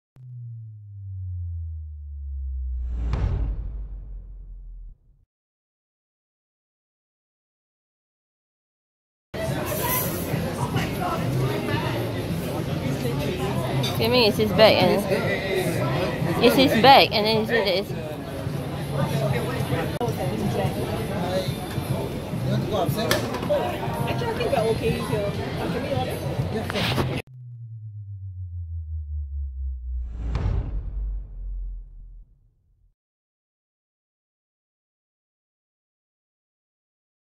Woman beats up man and takes haversack during dispute at Kampong Glam
A woman was arrested on Friday (Jan 5) after assaulting a man during a dispute over a haversack.